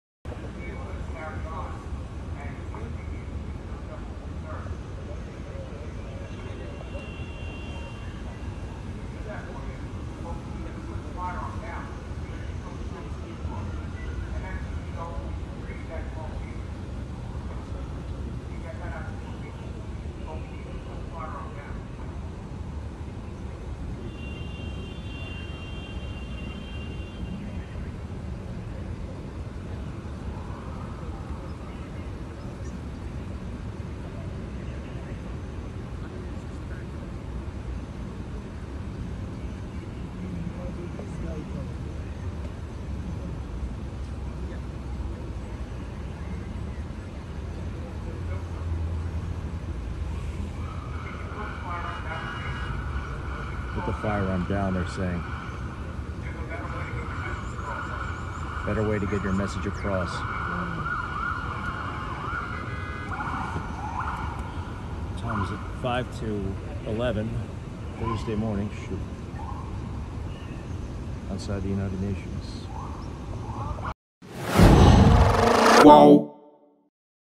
ONU: Sede das Nações Unidas é isolada em Nova York após ameaça de homem com arma
A sede da ONU (Organização das Nações Unidas) em Nova York foi isolada no final da manhã de hoje, após a polícia ser avisada sobre a presença de um homem segurando uma arma do lado de fora do prédio. "A sede da ONU está fechada, há atividade policial", declarou Stéphane Dujarric, porta-voz das Nações Unidas, à AFP.

Imagens compartilhadas nas redes sociais mostram policiais armados cercando o suspeito em uma calçada, se protegendo atrás de uma van, enquanto ele segura o que parece ser uma arma, não identificada.